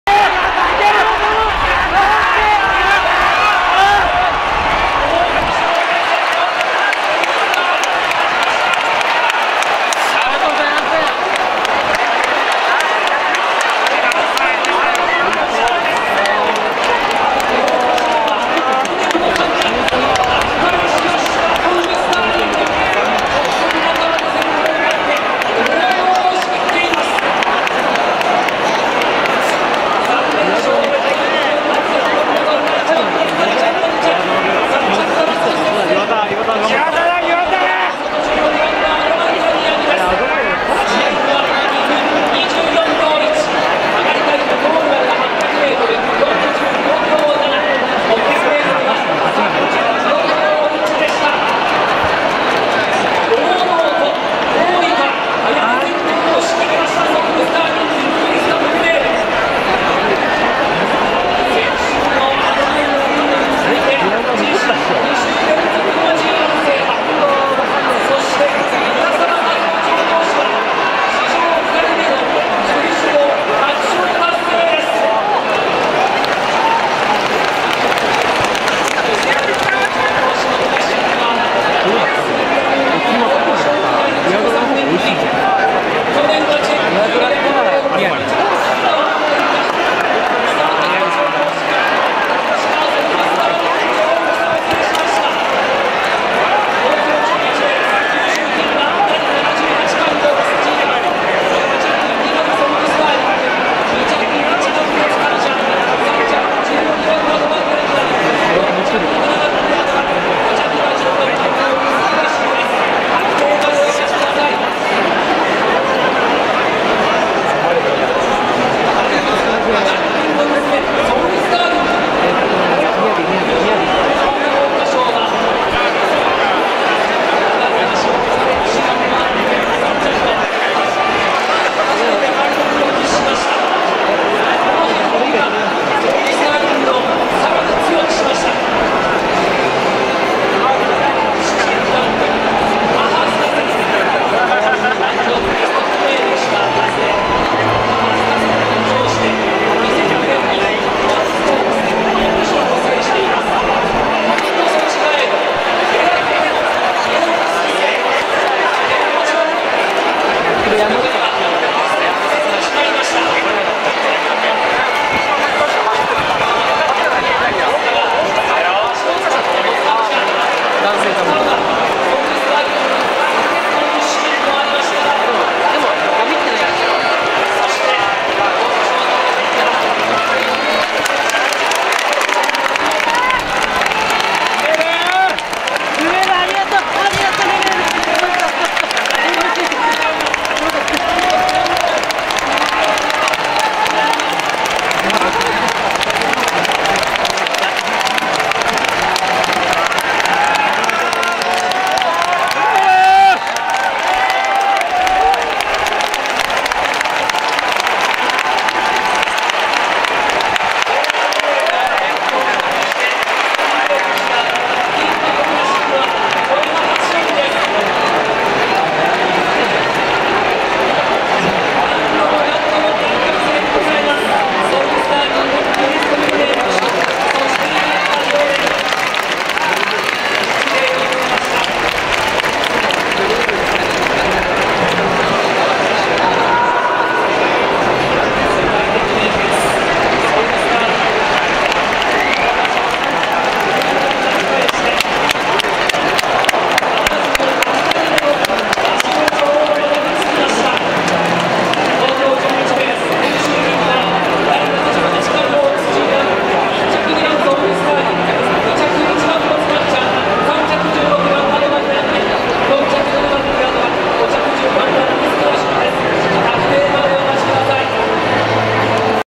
2017.05.21オークス優駿牝馬(G1)ウイニングラン(ソウルスターリング＆ルメール)＠東京競馬場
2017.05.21第78回オークス優駿牝馬(G1)ウイニングラン(ソウルスターリング＆ルメール)＠東京競馬場